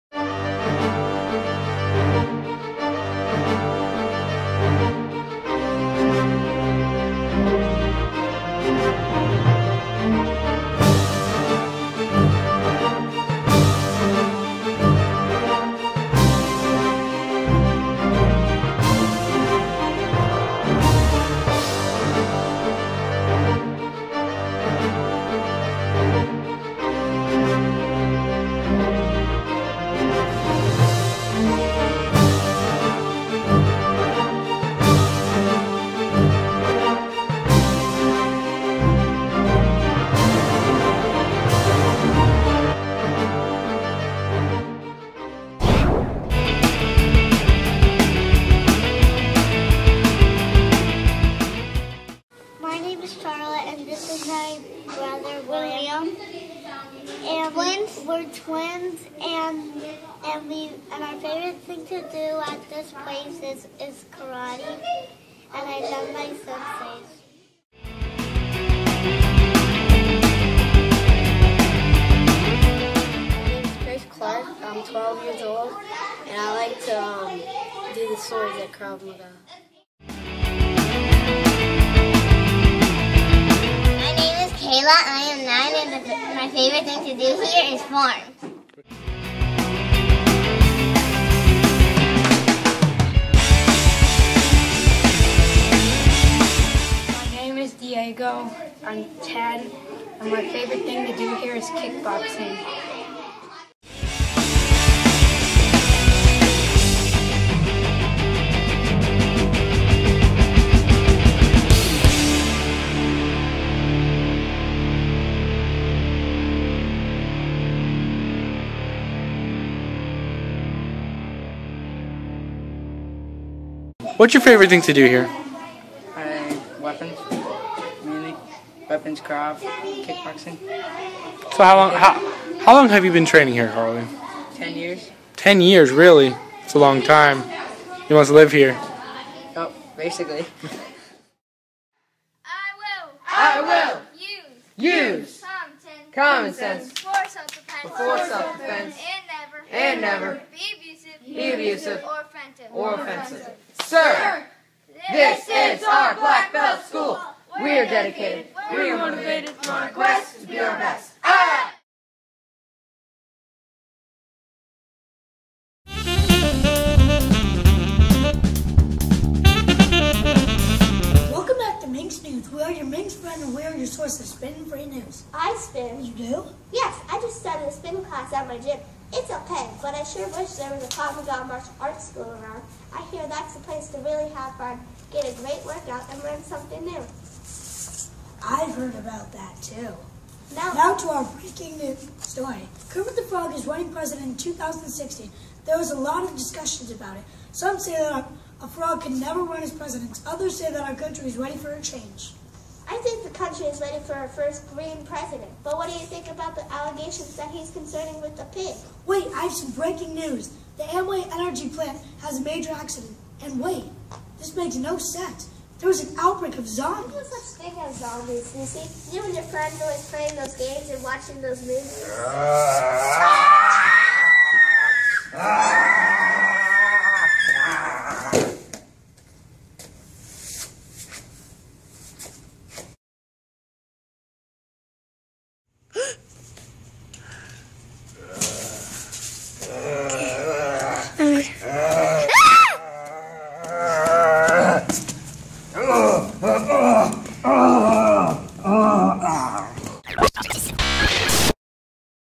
Defeating the Zombie Apocalypse Scene 1
In a land very close to home, there is a place where confidence is gained and discipline is taught. This place is called Krav Maga Martial Arts. No summer should pass without fun and excitement, and these kids chose to spend their summer having fun, learning something new, and making new friends. However, this group of unique, young martial artists will have to put their new-found Krav Maga skills to the test as they face the zombie apocalypse!